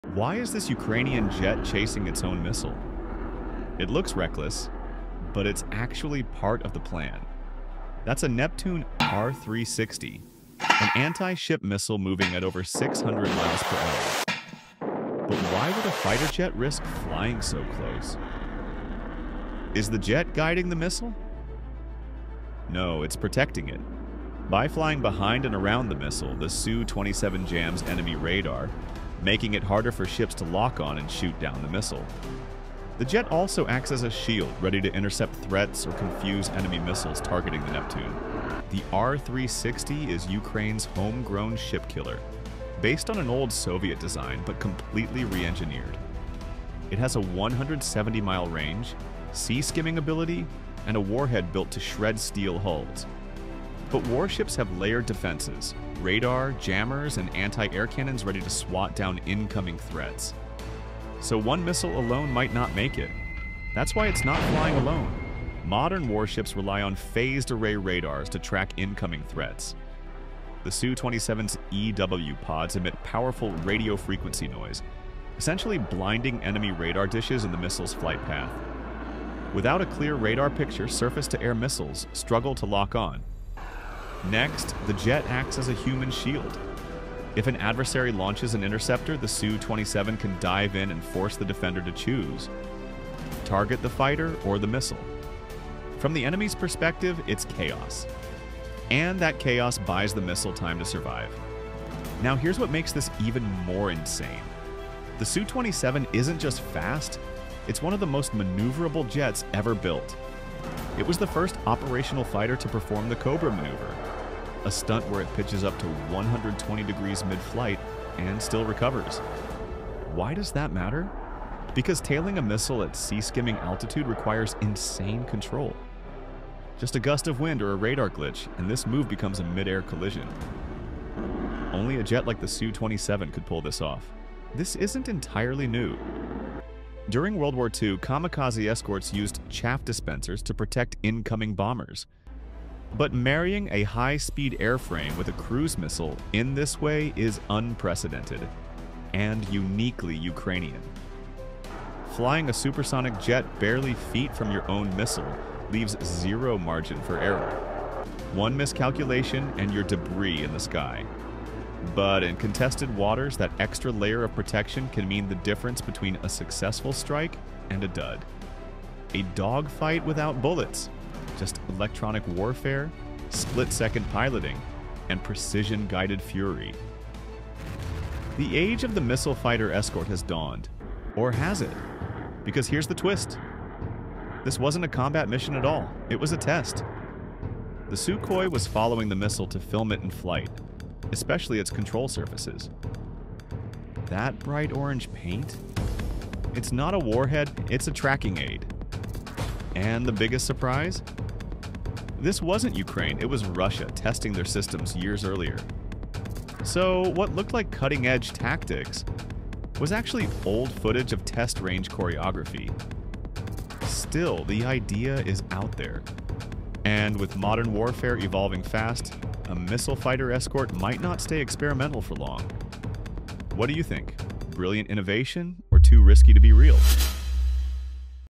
Why Is This Jet CHASING Its Own MISSILE?
Why would a fighter jet follow a missile just feet away at breakneck speed?
It looks like a bold Ukrainian tactic—an Su-27 shadowing a Neptune R-360 missile across the sky, protecting it from radar and interceptors. But here’s the twist: this wasn’t war—it was a test. And not by Ukraine... but by Russia.

In this video, we unpack the viral footage, explore how fighters can escort missiles, reveal the purpose behind this high-stakes maneuver, and break down the real engineering behind it all.
From missile defense to flight dynamics, this story is full of surprises—and one risky decision that might shape the future of warfare.